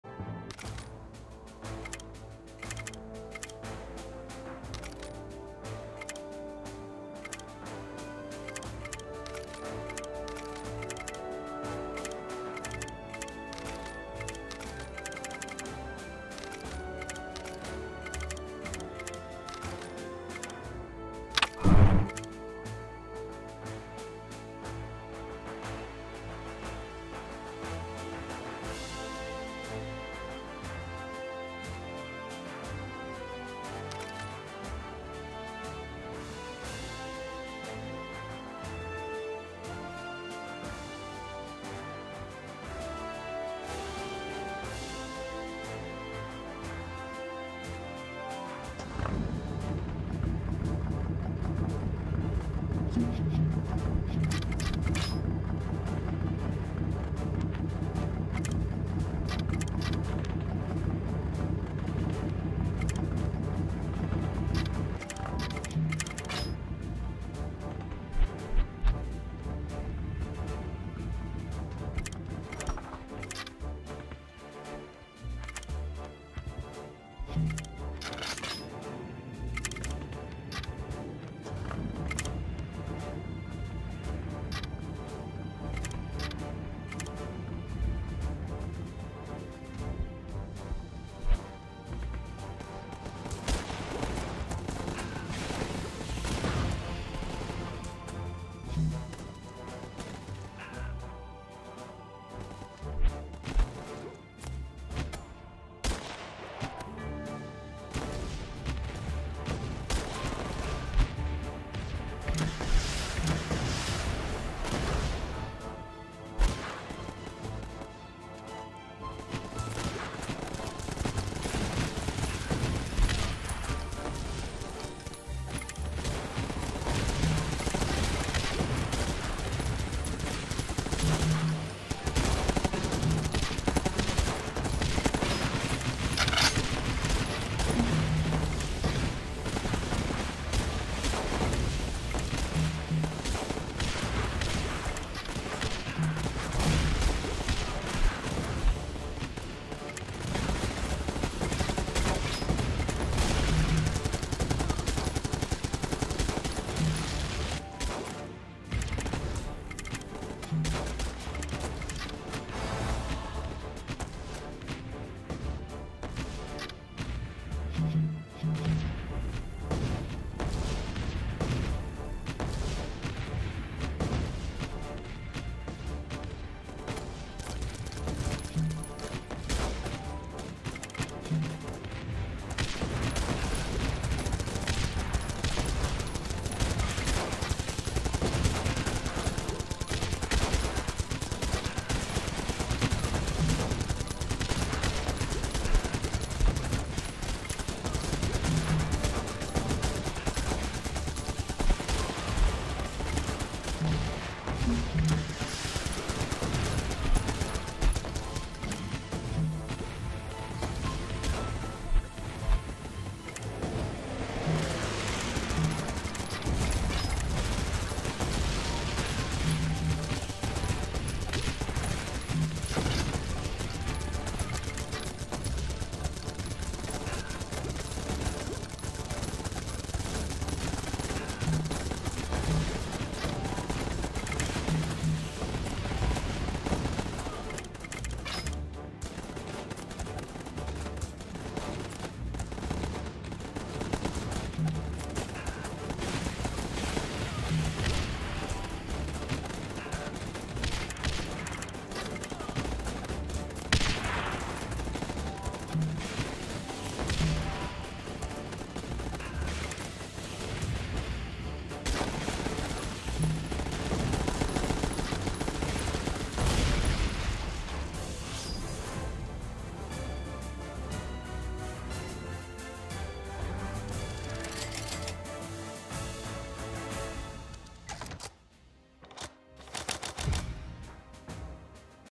3 Red skulls - First Foe
I had just unlocked 3 Red Skulls at L30 and my first component was L35